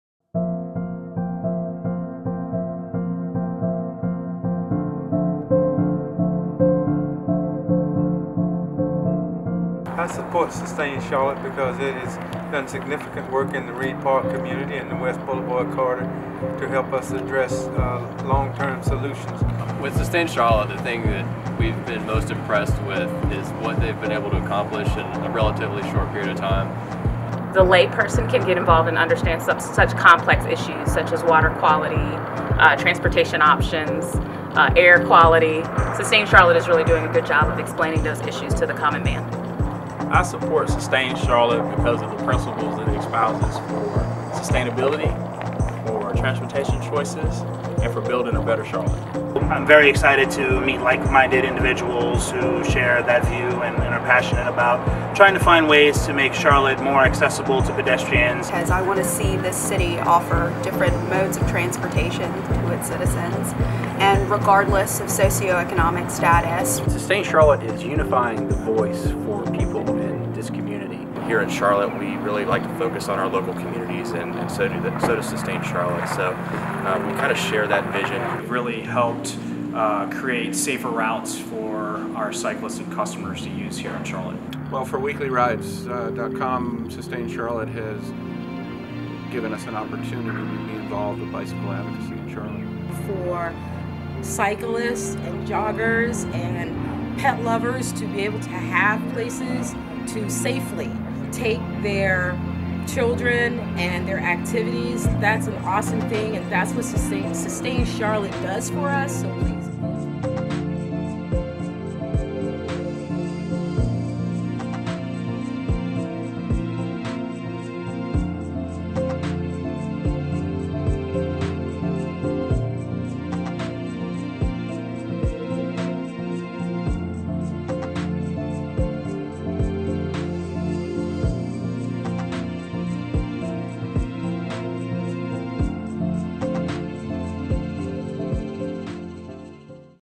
About Sustain Charlotte: Our Mission and Impact
Watch this short video to learn more about Sustain Charlotte, our supporters, and our impact in the Charlotte region!